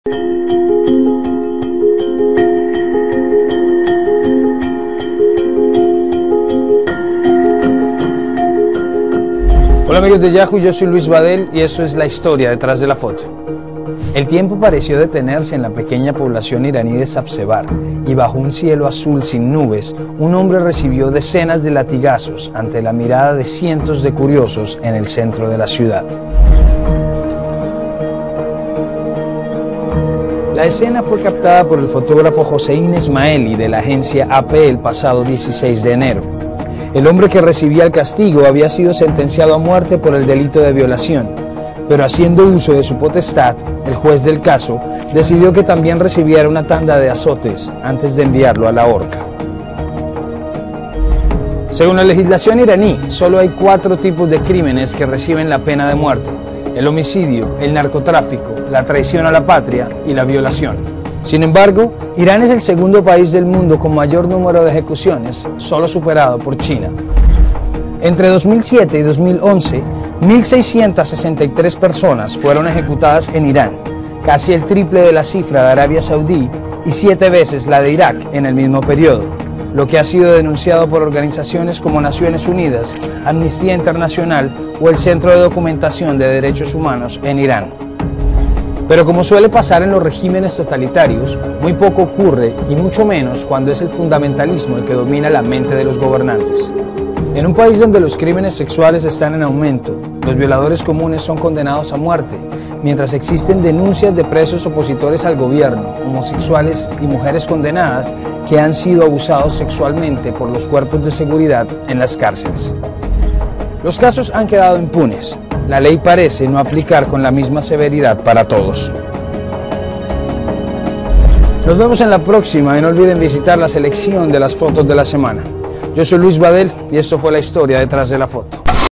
Doble Castigo
La historia detrás de la foto: Ante la mirada de cientos de curiosos en una plaza pública, un condenado a muerte cumple su última penitencia antes de ser ahorcado.